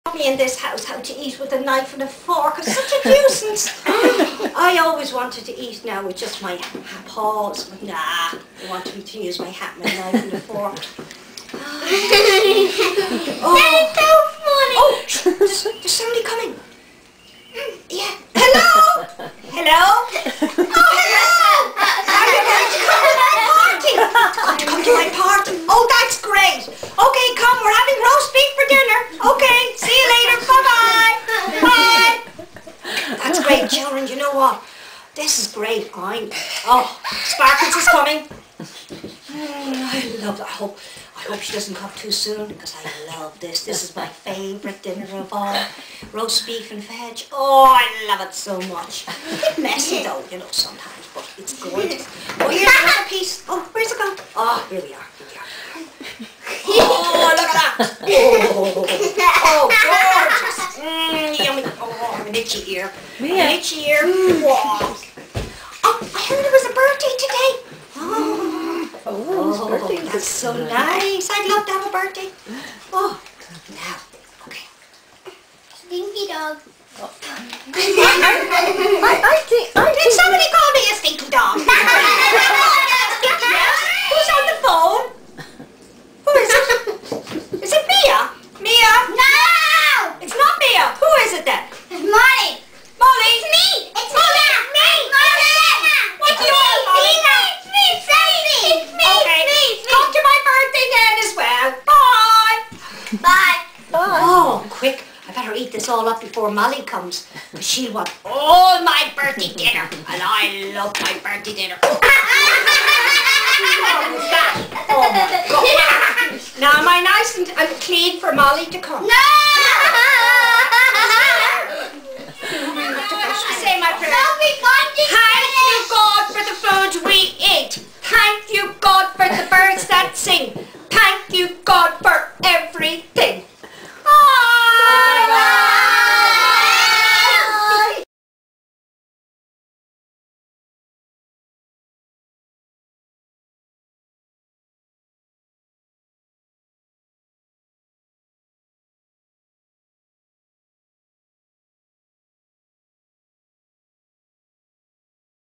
Puppy Birthday Party Show at Ylake Art
Mia enjoyed her Birthday puppet show starring Lexi the dog,